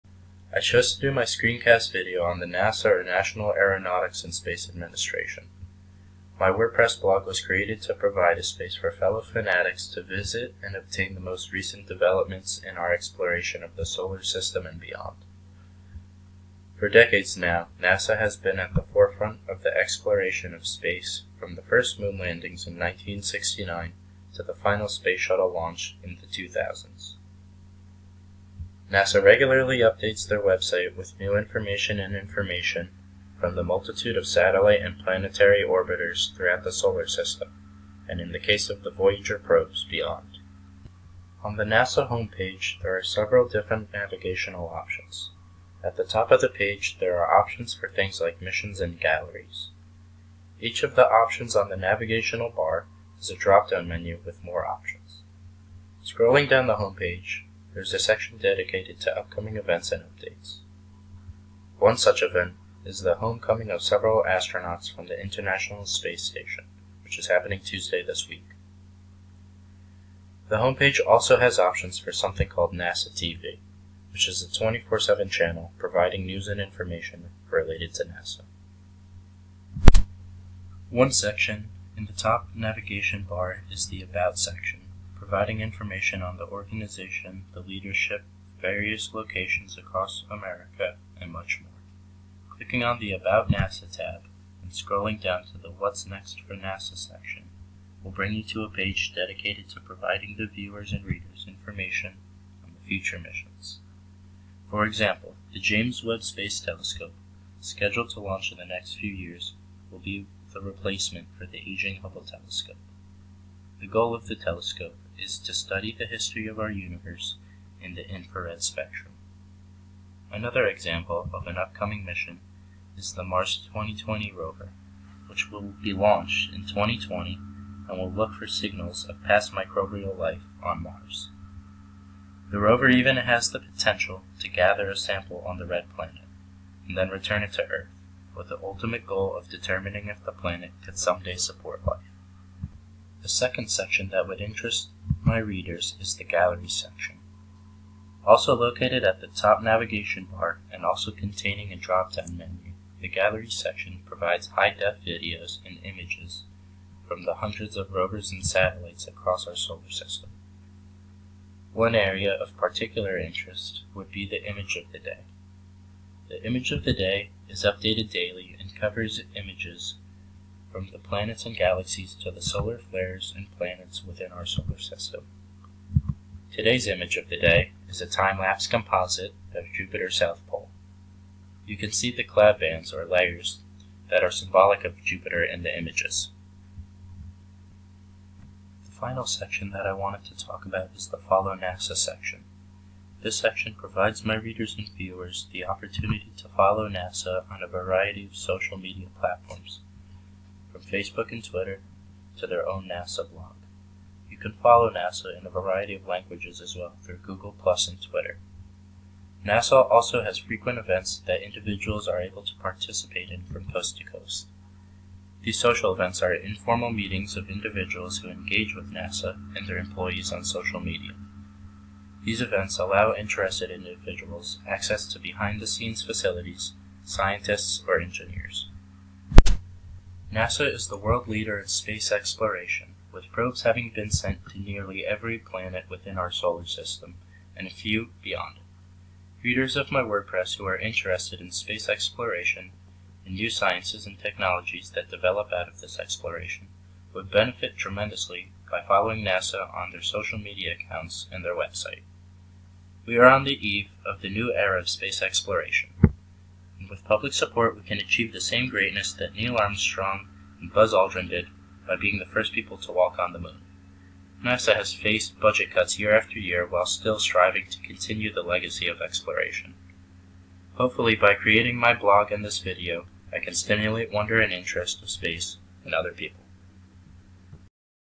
A short visit @ NASA.gov
This is my short video detailing a few key areas that readers of my blog (linked below) would find interesting and illuminating.  NASA is the National Aeronautics and Space Agency, headquartered in Florida, but with locations across America.
NASA is very good at updating their website with information, pictures and details of current and upcoming missions.